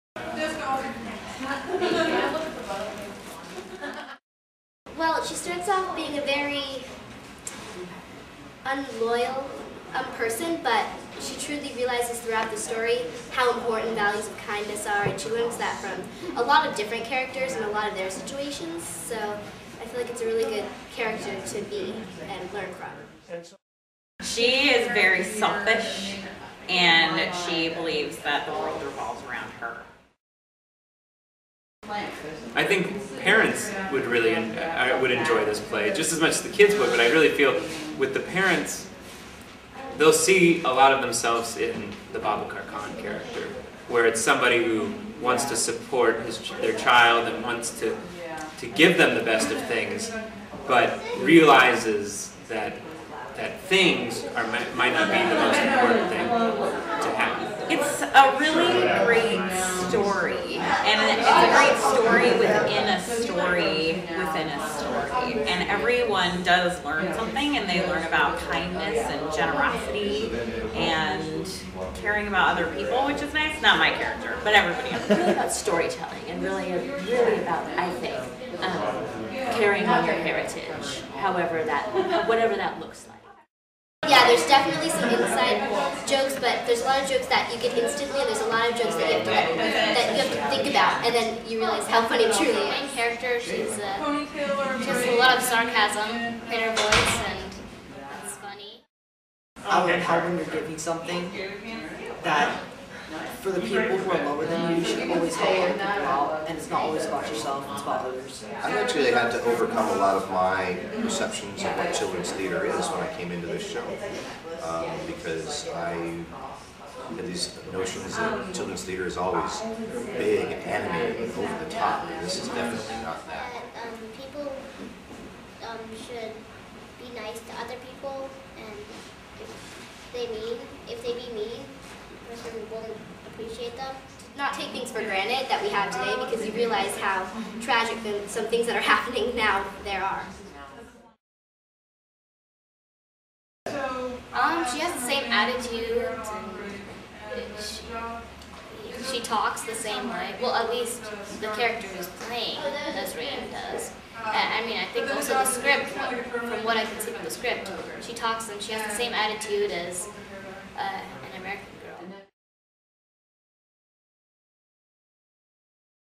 Talking to the cast of Eve
Talking to the cast of On the Eve of Friday Morning during one of their last rehearsals before moving into the theater. The chatter in the background is the costumer and director doing final costume fittings. On the Eve of Friday Morning is an original play by Norman Allen.

Feb 26 - Mar 20
Winningstad Theatre

This is a magical story about the power of stories themselves, brought to the stage in a tapestry of color and light. Nassrin is a young girl in modern Iran whose father has just been arrested for smuggling banned books. To ease Nassrin's fears, her mother begins to tell a story, one as ancient as time. Concurrently, Bahad, an orphan boy from a thousand years past, listens as a wandering storyteller narrates the same tale. The two worlds, separated by the ages, intertwine as Nassrin and Bahad step into the story and meet a poor twig-gatherer, a spoiled princess, and the mysterious and powerful Mushkil Gusha—the Remover of Obstacles.

Join Nassrin and Bahad, two children learning the universal values of kindness and generosity through ancient tales from their rich Persian culture. Lyrical and magical, this play will bring you hope. Best for ages 8 and up!